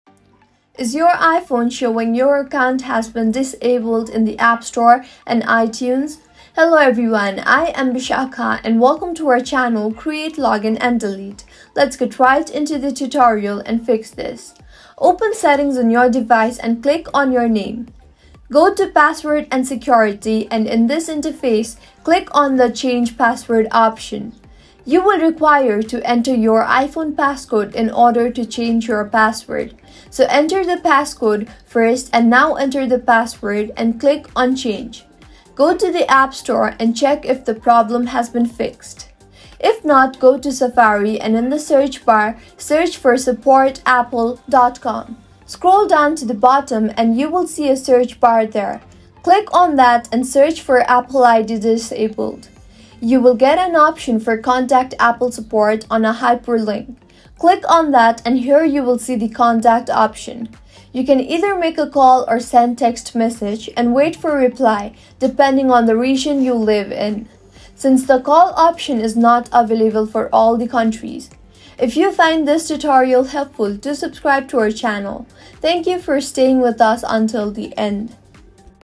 How To Fix Your Account Has Been Disabled In The AppStore and iTunes 2023
In this tutorial, we'll provide you with a step-by-step guide on how to resolve the issue of your account being disabled on the App Store and iTunes. We'll cover everything from understanding the possible reasons for the account disablement to methods for restoring your account access.

👋 Welcome to Create Login And Delete! In this video, we'll guide you through the process of fixing the issue of your account being disabled on the App Store and iTunes. If you're facing problems accessing your account or making purchases on these platforms, this tutorial is perfect for you.

Tips for fixing "Your Account Has Been Disabled" on the App Store and iTunes:
🔗 If you have any questions or need assistance with fixing your disabled account on the App Store and iTunes, please leave a comment below, and we'll do our best to assist you.

📱 Timestamps:

0:00 - Introduction
0:05 - How to Fix "Your Account Has Been Disabled" in the App Store and iTunes
0:10 - Steps to Fix "Your Account Has Been Disabled" in the App Store and iTunes